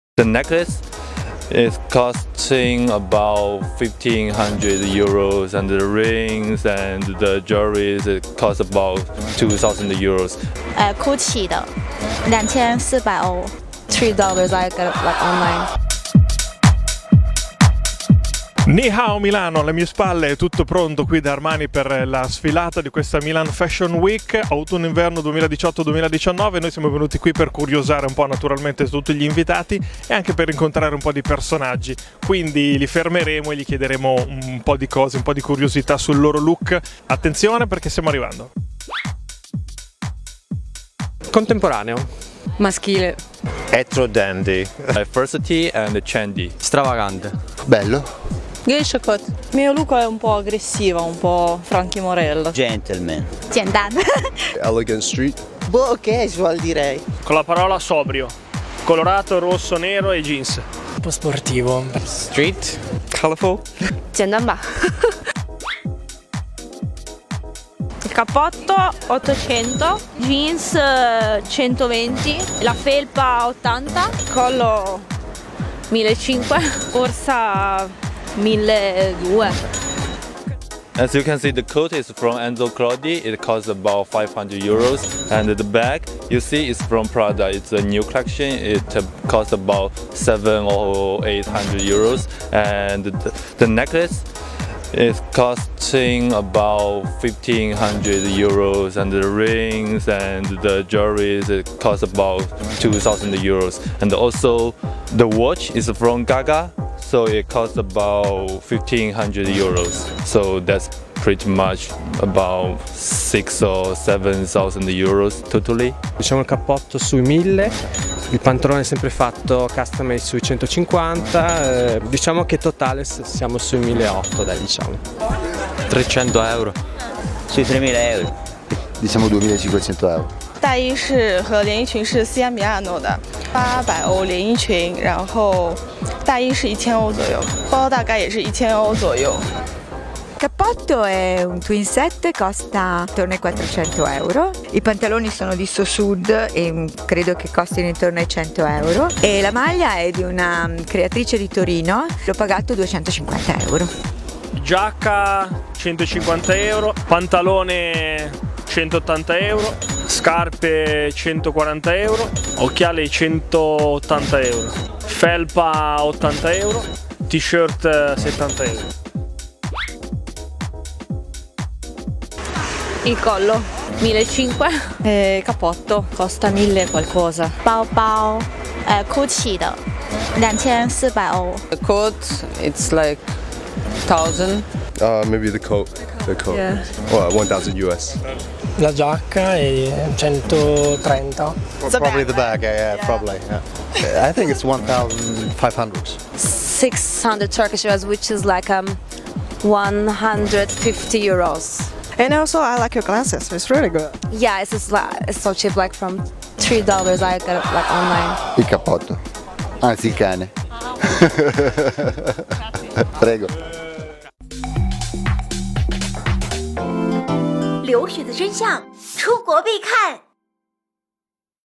【留学Tube】混时尚圈的都是有钱人吗？米兰时装周嘉宾一身值多少钱？[原画版]
留学的真相，不一样的故事，打破信息不对称。